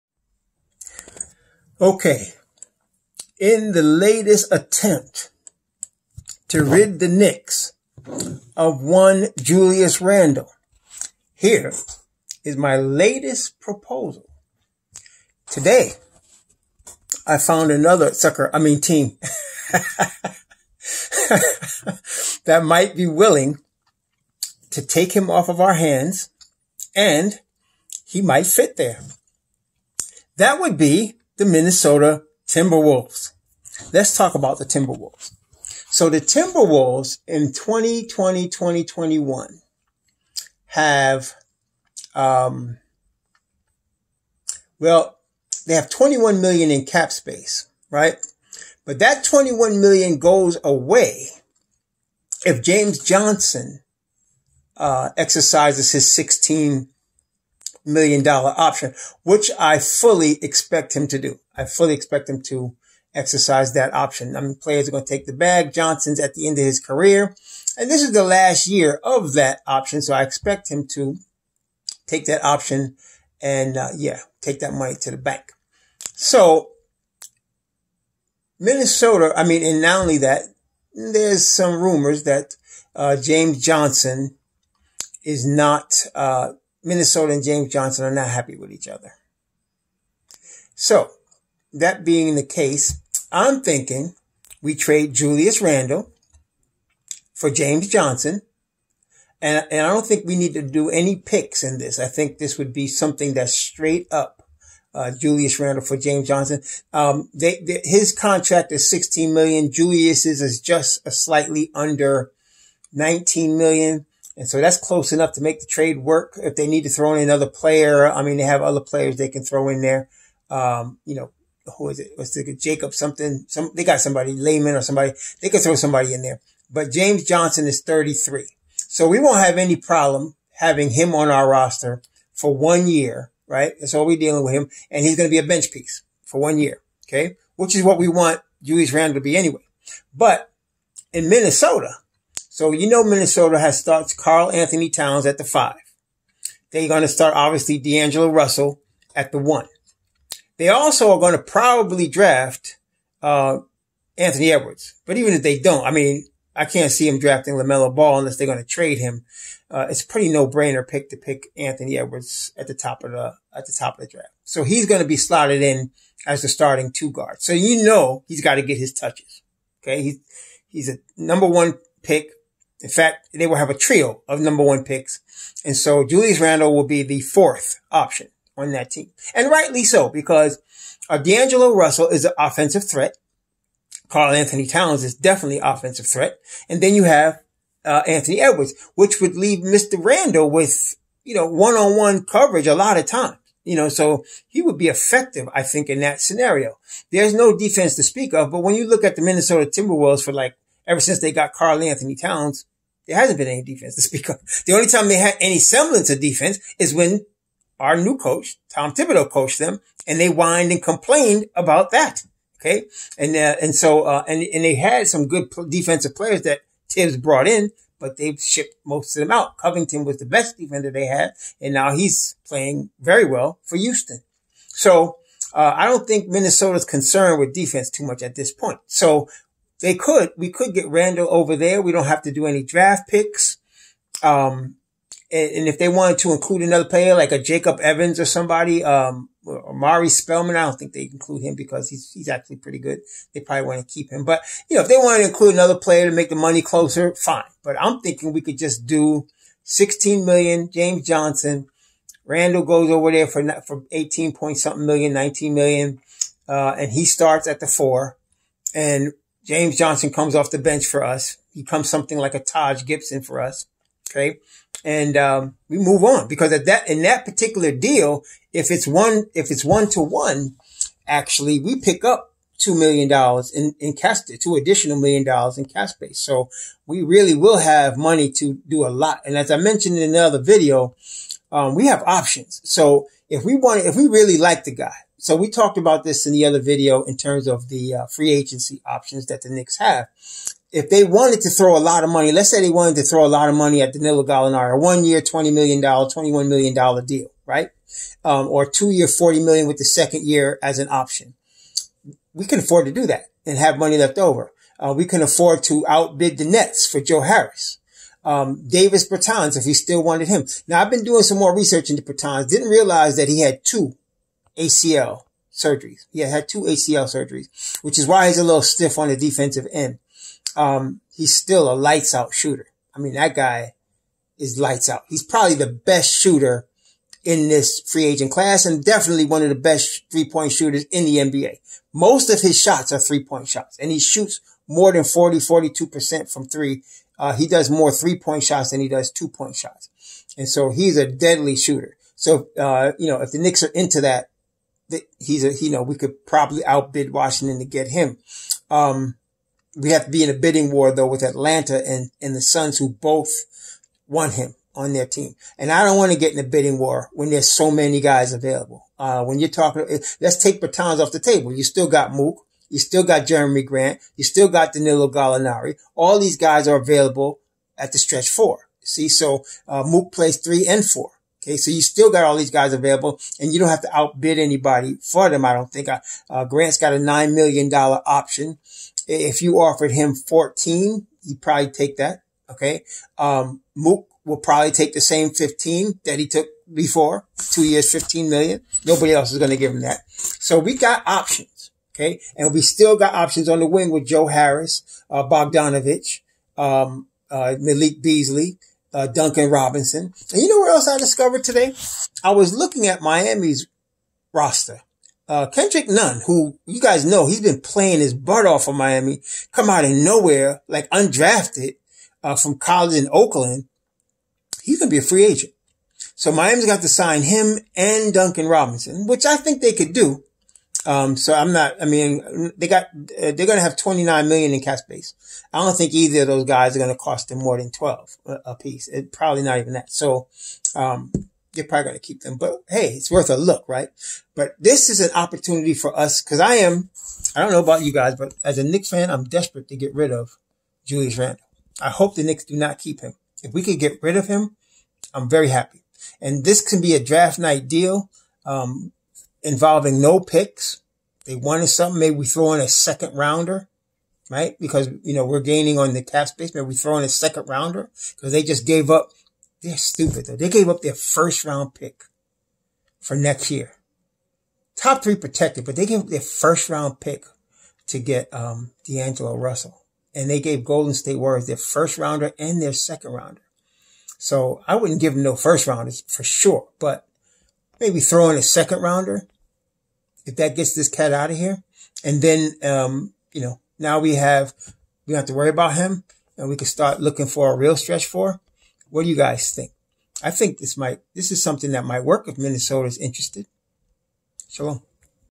How the Knicks can trade Julius Randle
Getting rid of Randle is addition by subtraction